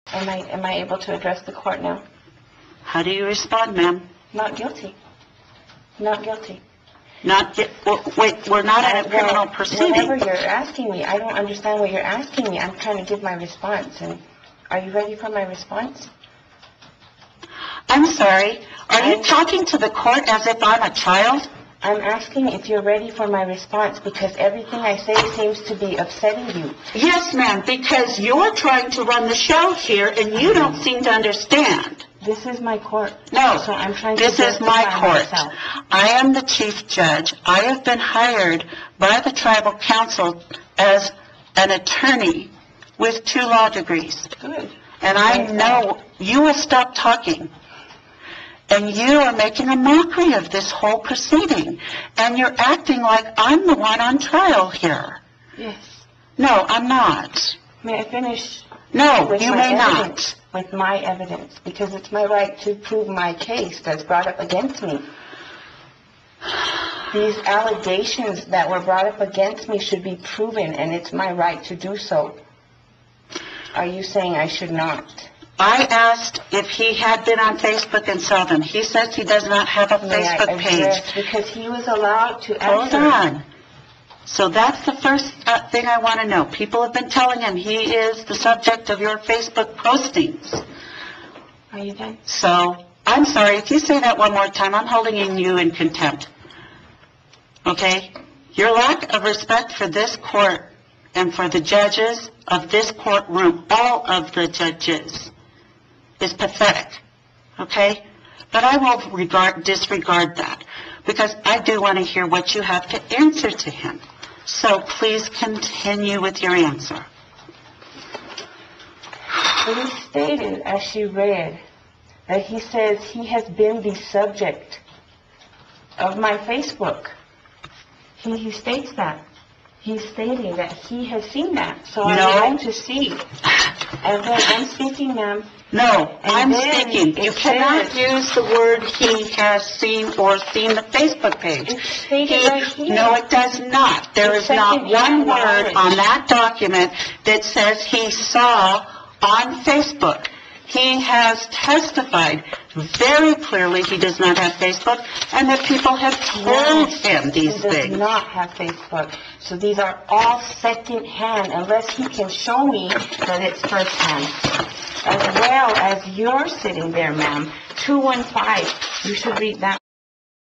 Mescalero Apache drug dealers running casino @InnoftheMtGods go after Apache woman when asked
Mescalero Apache cocaine drug dealers go after Apache woman try to harm her family.